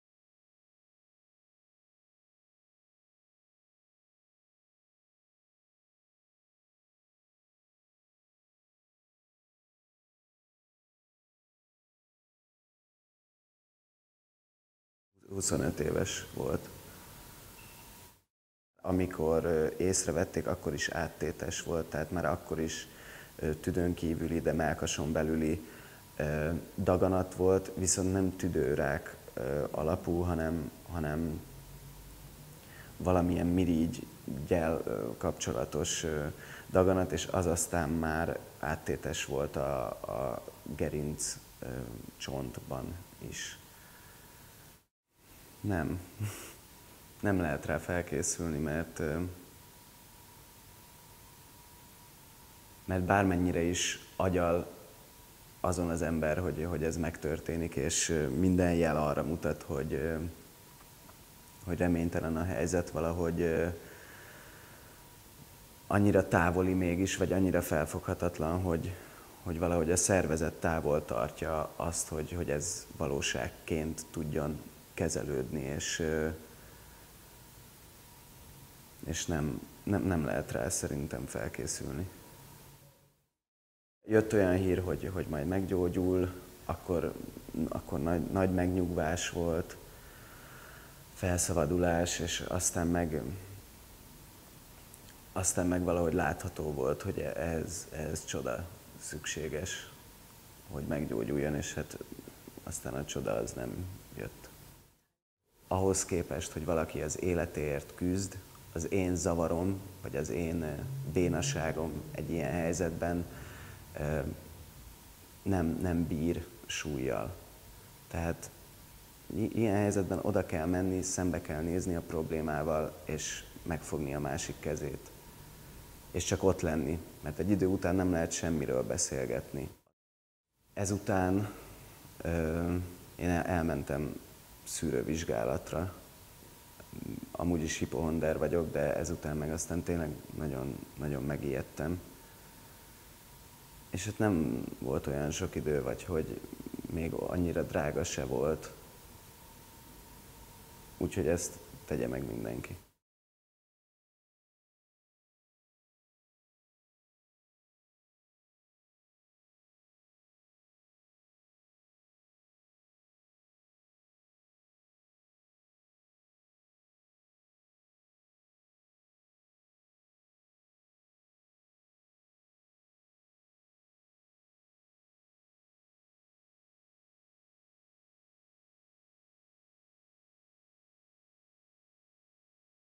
Nézőpontváltó - Radnóti Színház
A Radnóti Színház színművészei, alkotói és munkatársai országos figyelemfelhívással fejezik ki a daganatos betegségek elleni küzdelem fontosságát. Köszönet Fehér Balázs Benőnek!